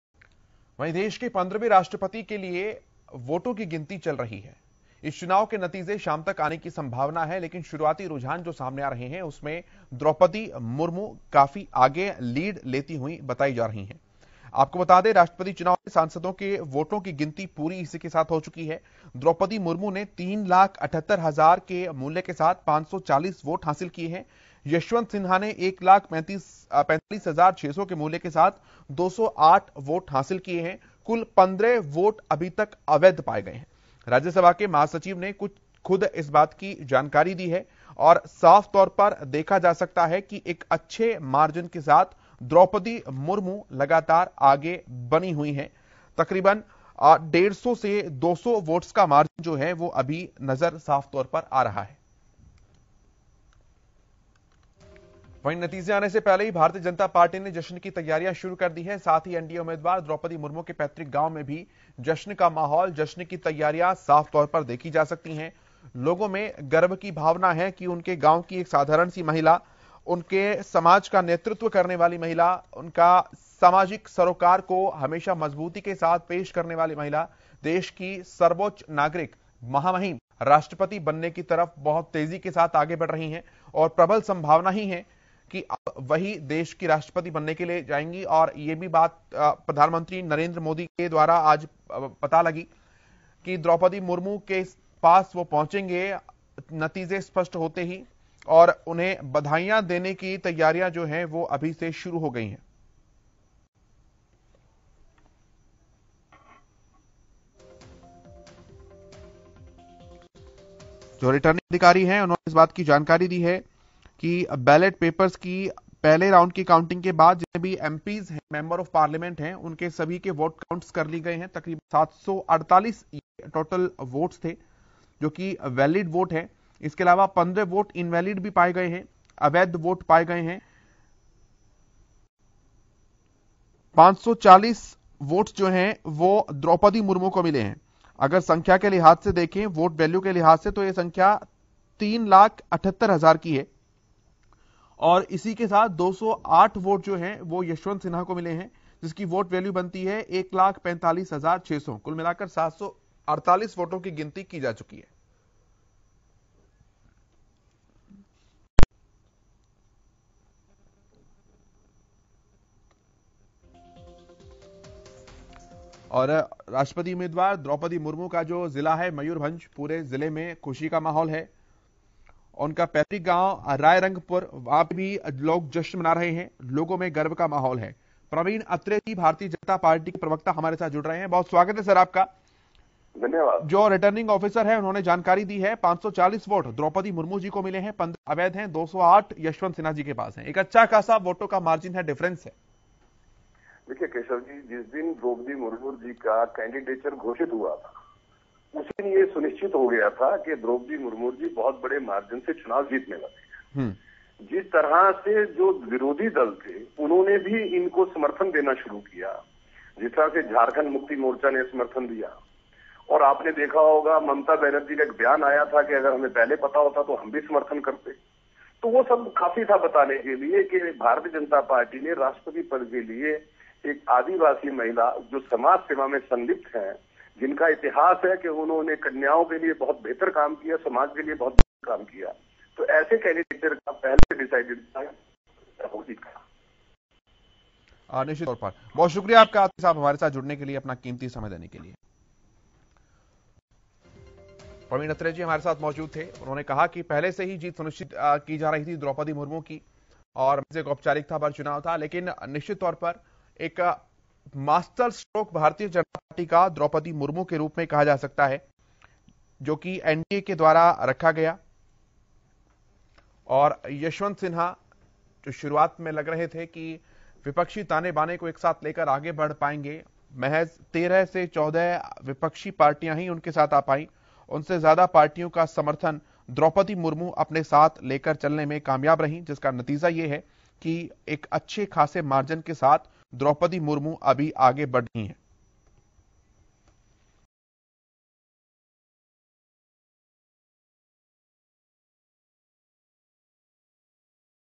देश को आज मिलेंगे नए ‘महामहिम’, किसके लिए सजेगा रायसीना हिल्स?
देश के 15वें राष्ट्रपति के चुनाव के लिए वोटों की गिनती जारी है. बता दें कि इस चुनाव के नतीजे शाम तक आने की संभावना है. कड़ी सुरक्षा के बीच संसद भवन परिसर में काउंटिंग की जा रही है.